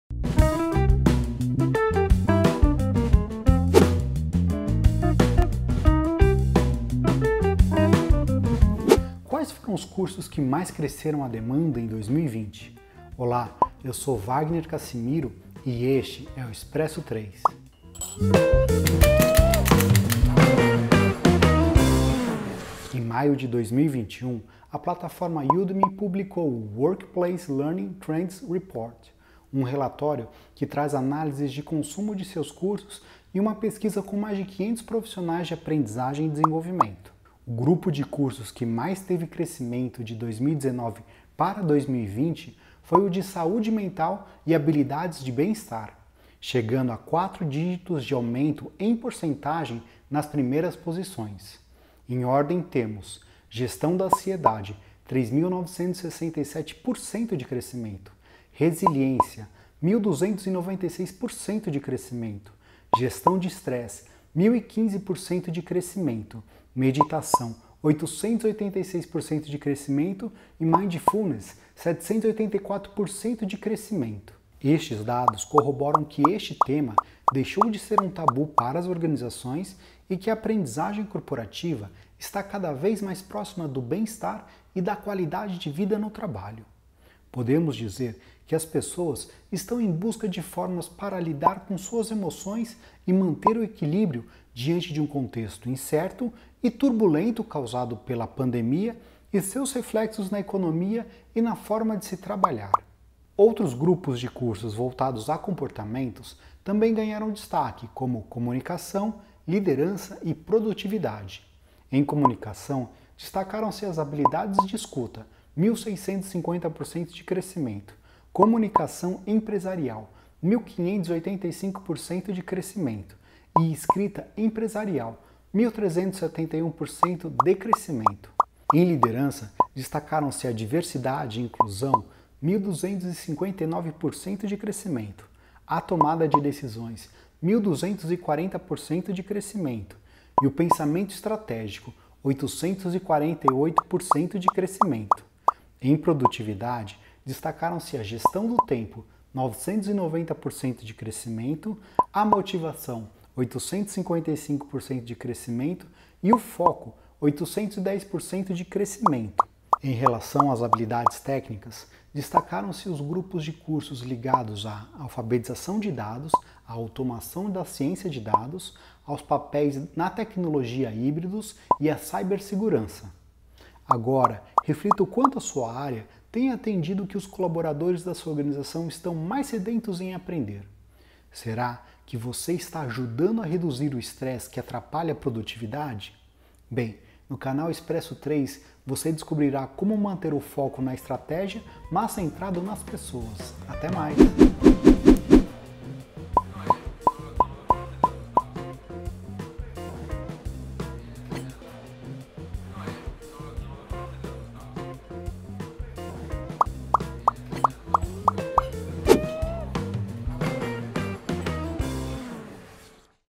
Treinamentos que mais cresceram a demanda em 2020
Em maio de 2021 a plataforma Udemy publicou o “Workplace Learning Trends Report”, um relatório que traz análises do consumo de seus cursos e uma pesquisa com mais de 500 profissionais de aprendizagem e desenvolvimento.

Saiba em nossa aula de hoje, quais cursos tiveram os maiores aumentos na demanda de 2019 para 2020.